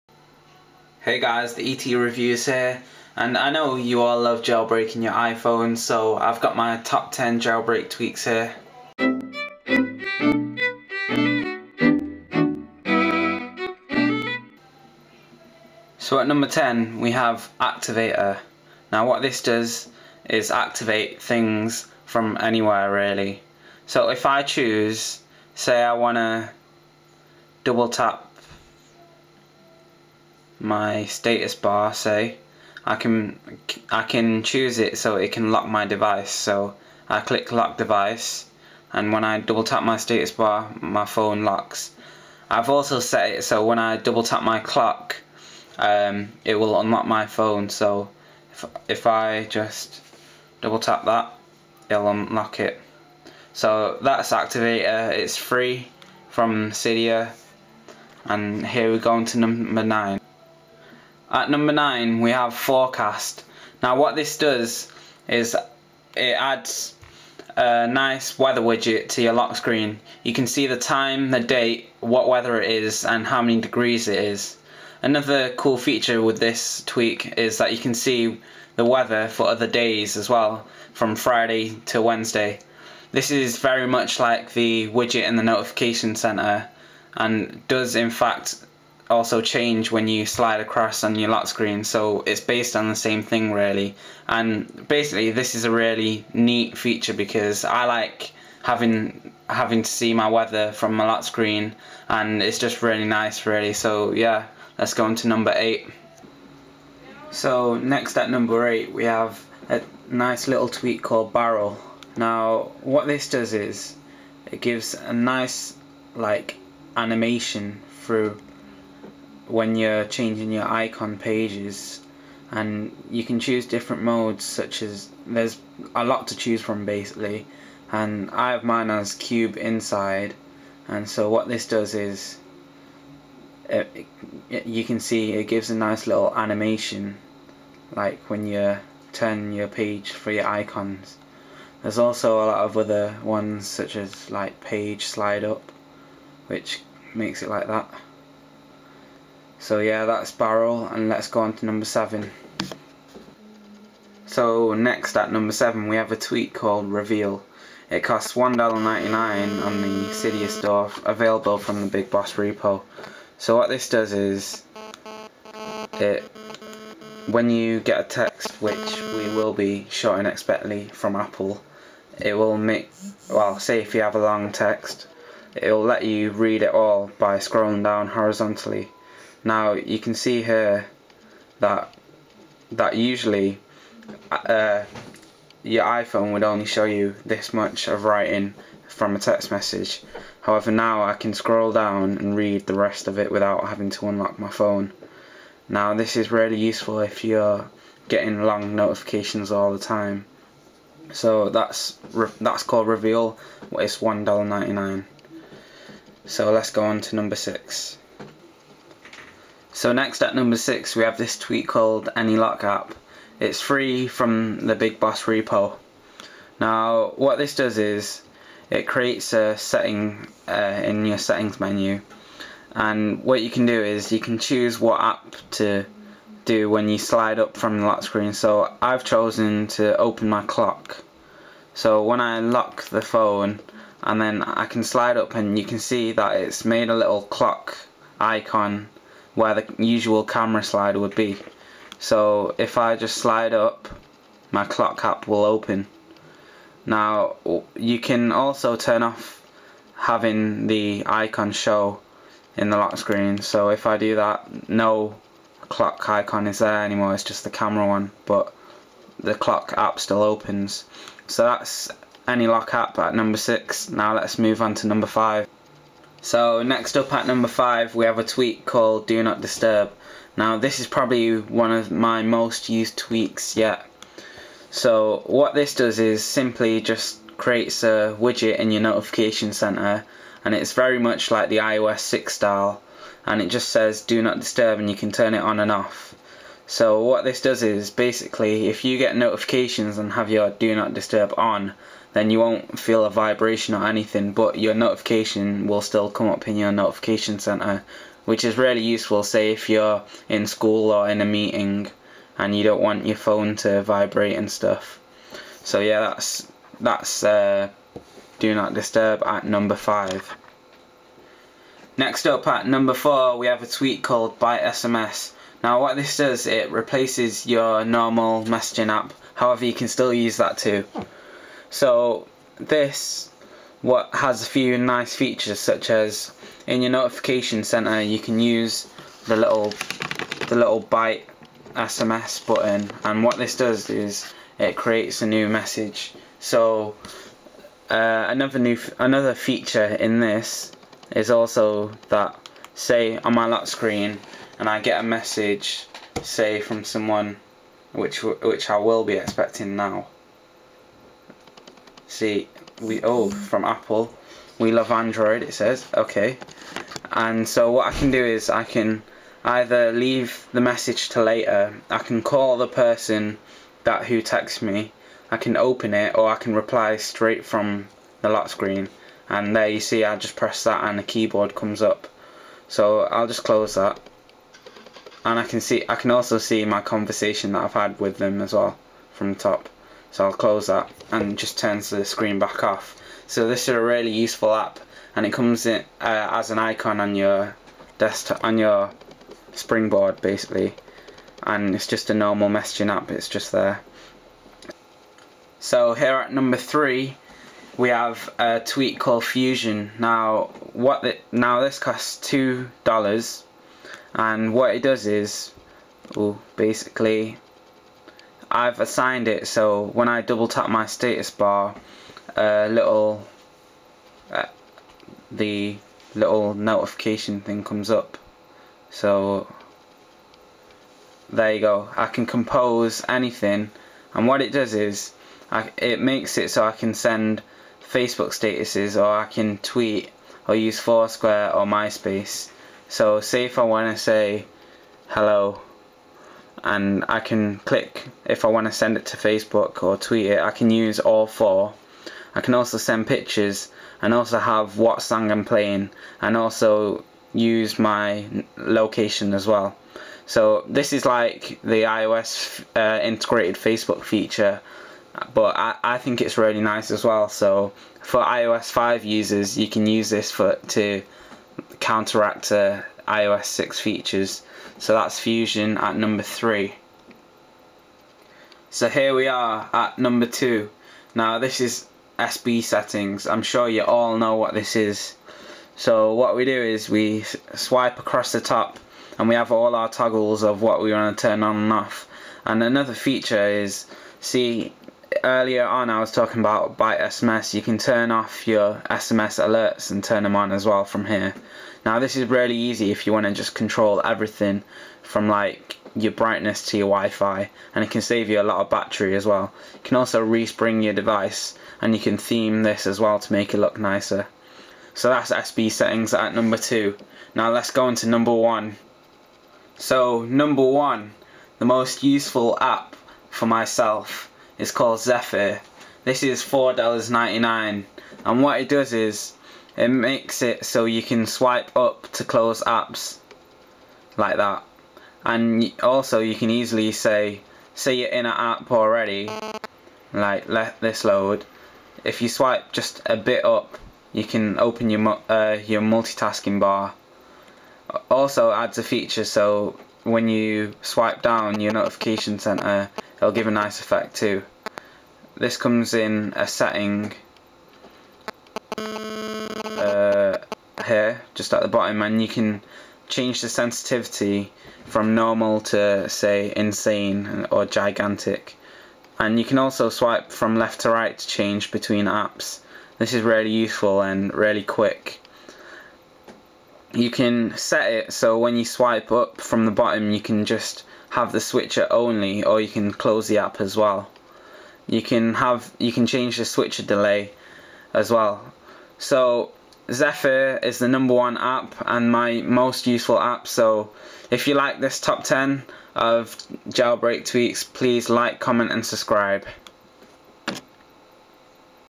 TOP 10 Jailbreak Tweaks of 2012
Here is a review of the top 10 jailbreak tweaks available for apple devices.